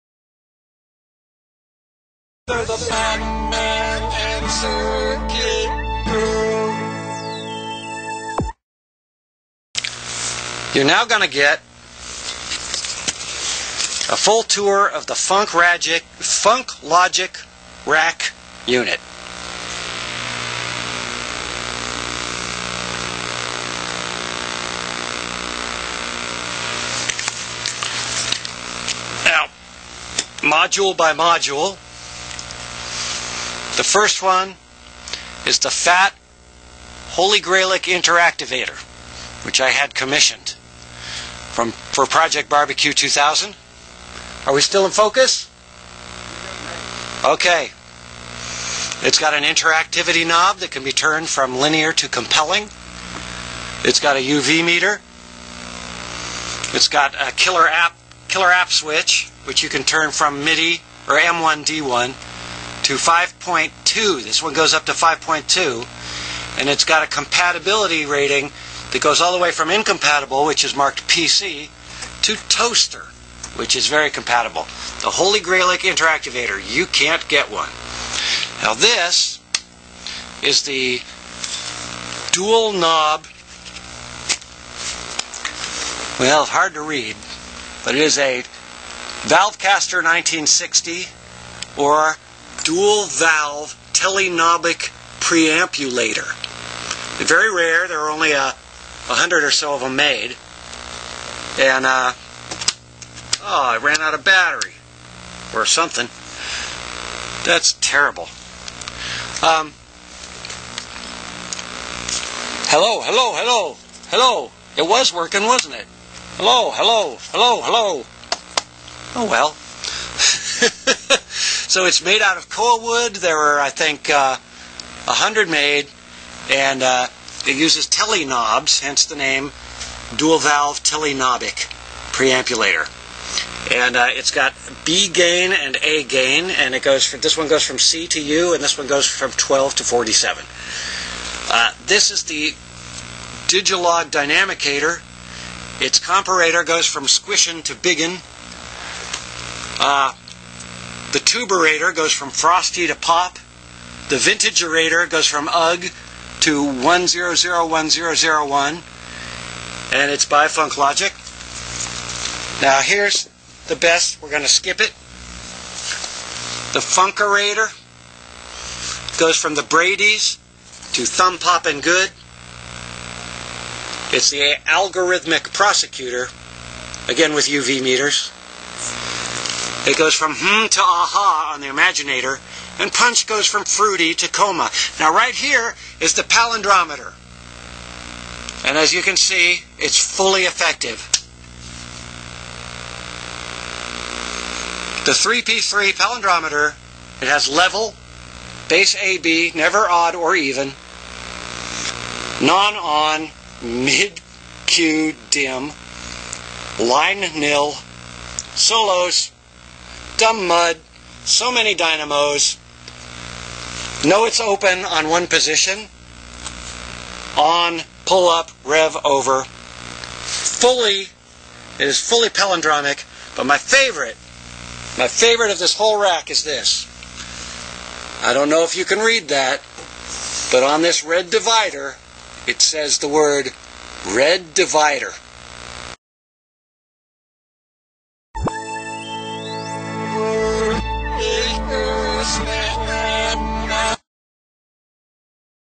03-01-10 03a funk logic rack.wmv
Custom rack of audio gear that is _really_ quiet.  See funklogic.com.